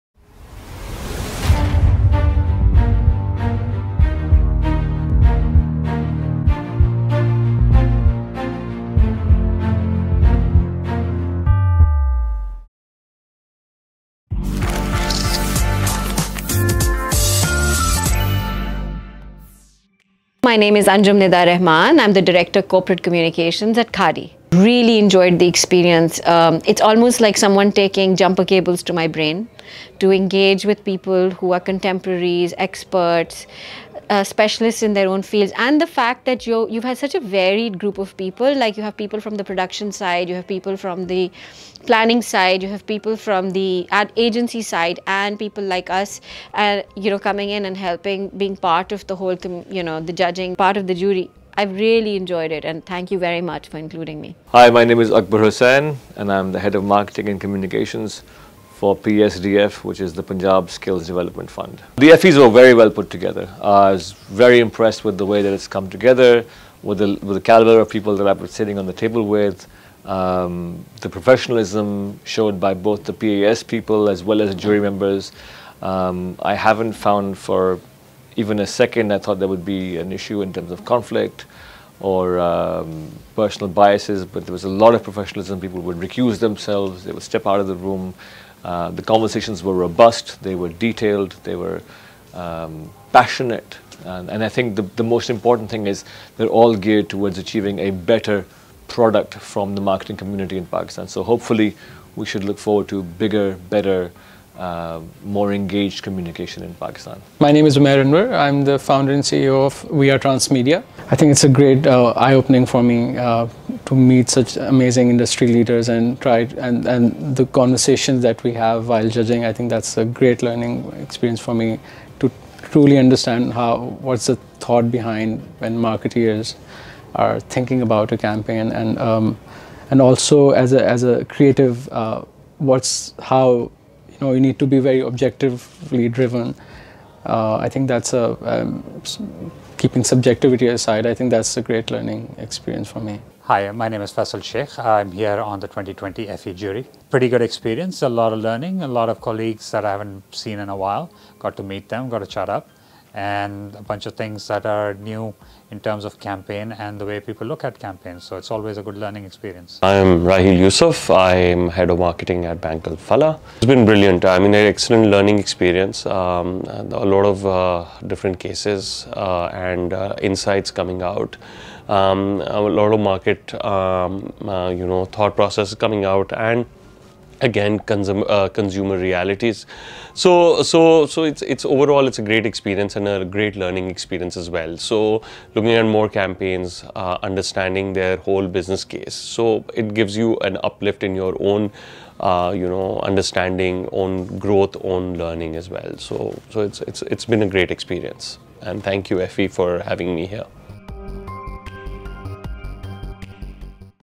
Effie Awards 2020 - In Conversation with Jury
Hear our jurors' experience on becoming a part of the Effie Awards jury for the first time.